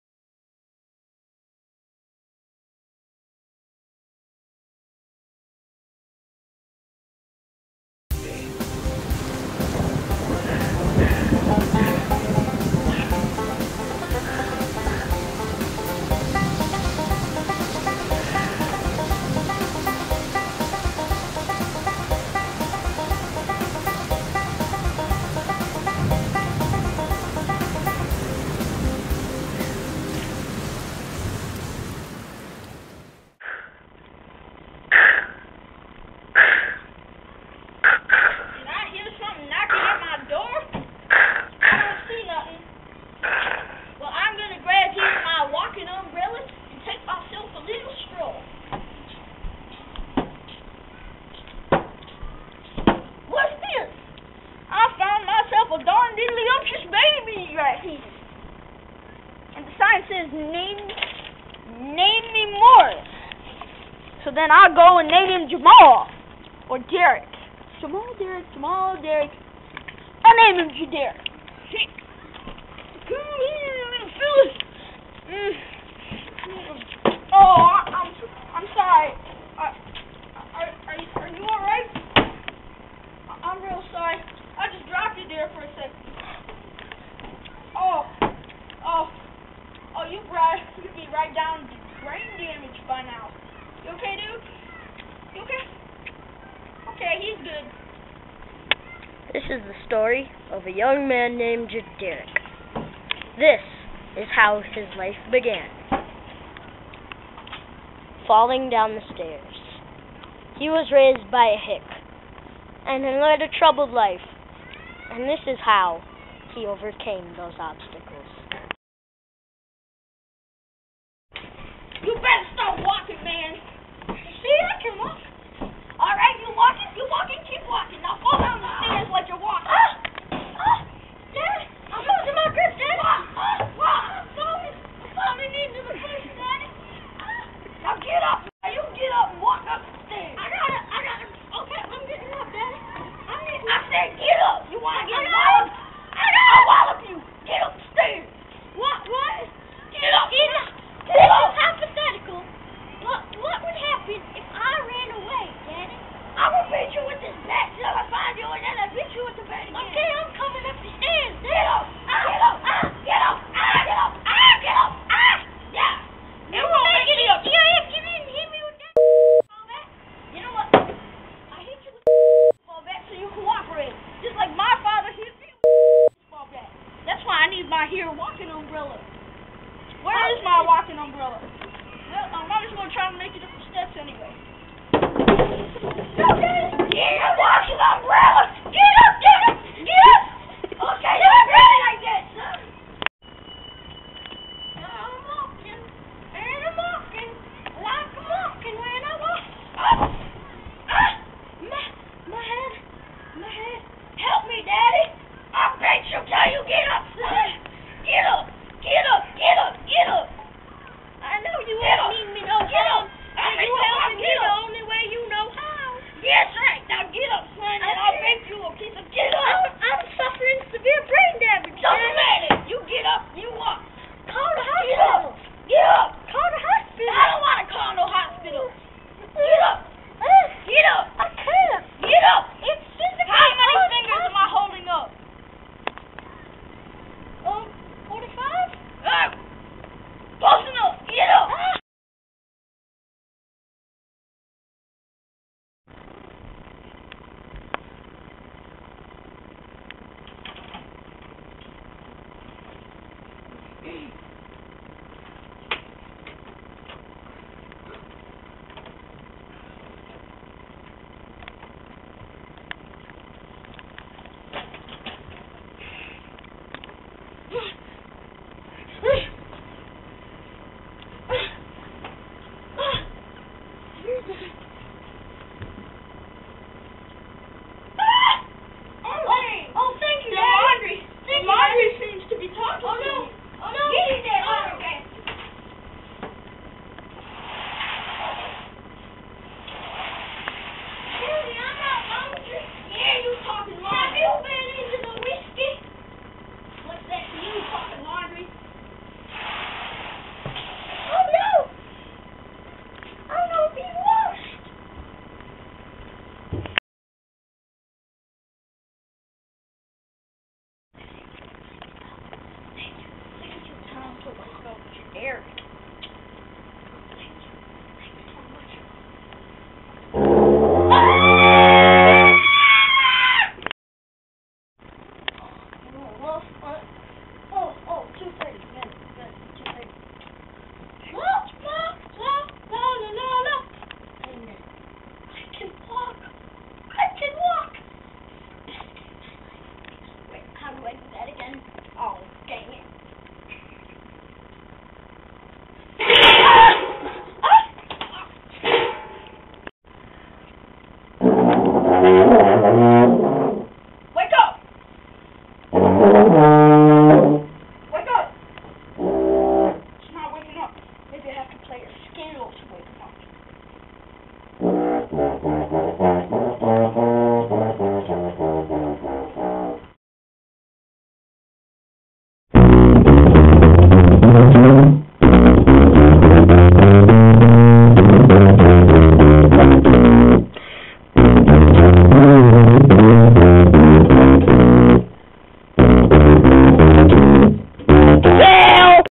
The Life and Times of Ja'Derrick
The story of a poor boy who was raised by a hick, and due to severe brain damage has to walk heel-to-toe backwards all the time.